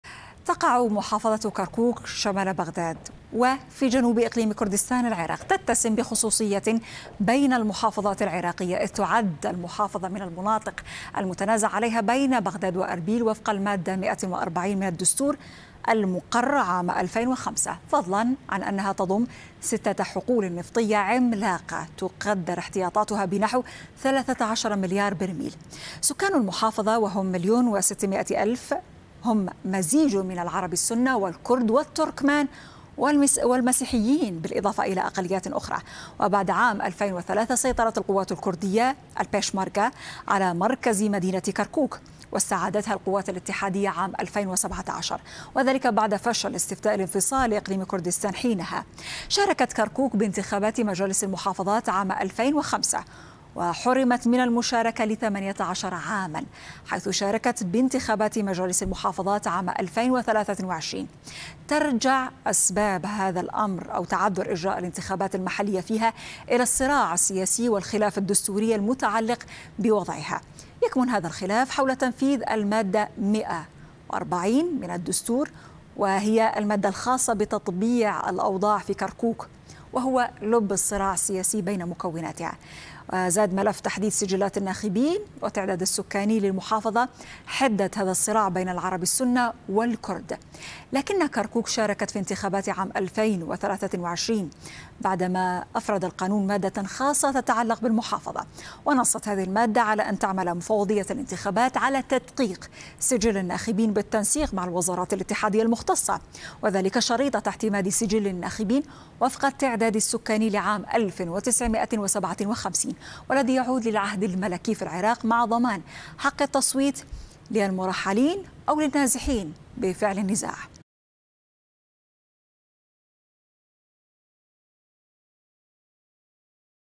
العراق.. كركوك نقطة نزاع بين بغداد وأربيل
بها 6 حقول نفطية تقدر احتياطاتها بنحو 13 مليار برميل.. كركوك من المناطق المتنازع عليها بين بغداد و